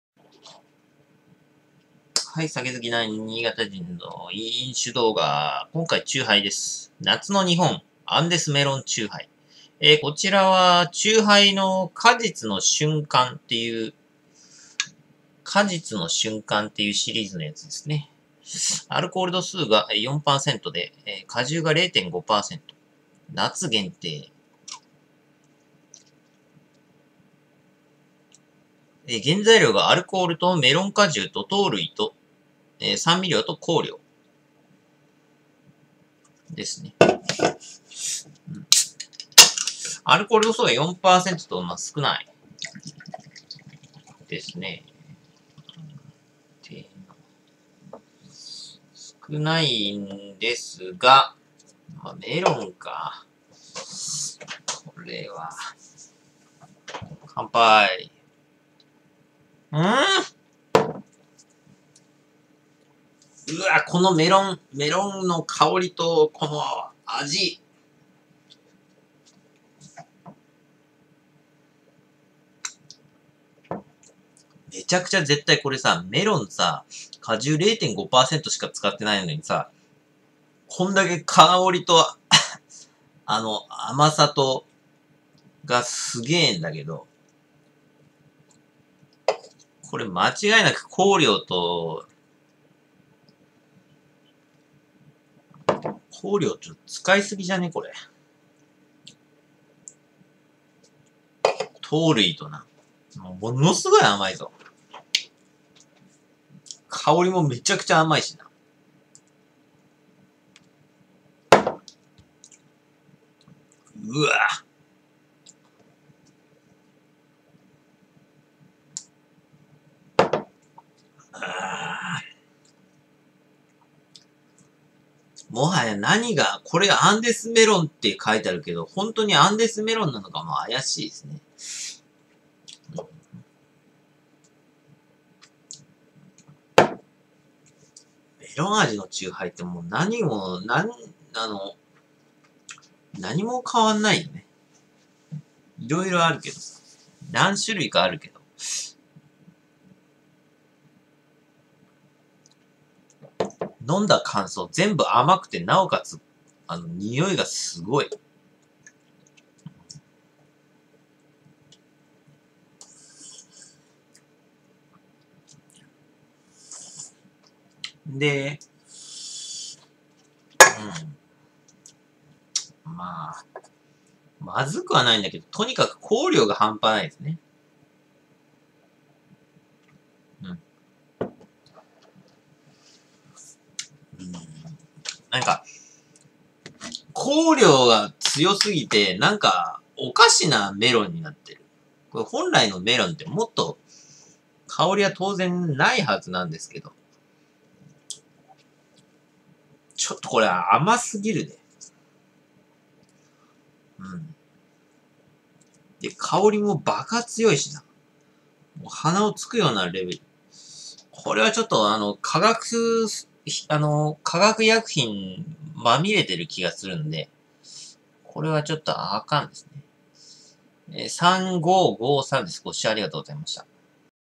酒好きな元新潟人の飲酒動画 part1370 果実の瞬間 アンデスメロン 【チューハイ】Japanese Drinking Beer Liquor
甘み：★★★★★
香り：★★★★★
買い？度：★★★
twitterが新しいアカウントになりました⇒@yukki_niconico2
LINEもやってます。良ければ申請してね⇒